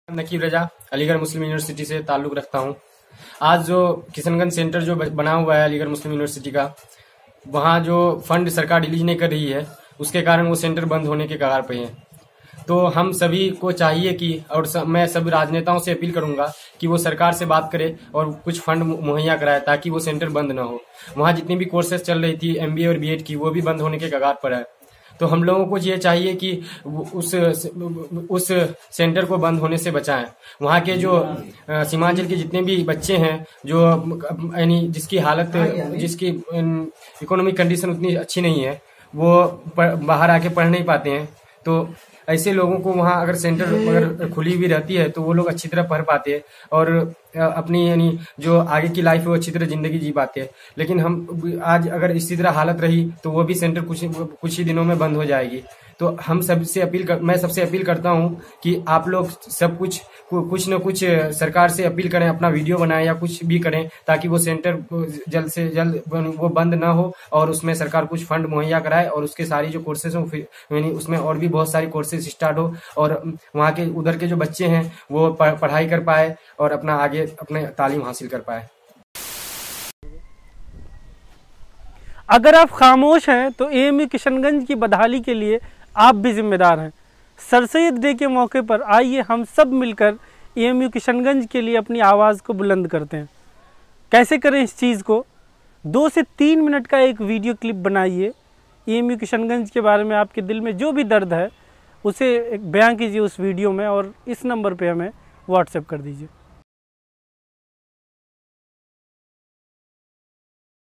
Save AMU Kishanganj | Video 028 | Naquib | AMU Aligarh
अलीगढ मुस्लिम यूनिवर्सिटी, अलीगढ के नकीब रज़ा बता रहे हैं क्यों एएमयू किशनगंज इलाक़े के लिए ज़रूरी है।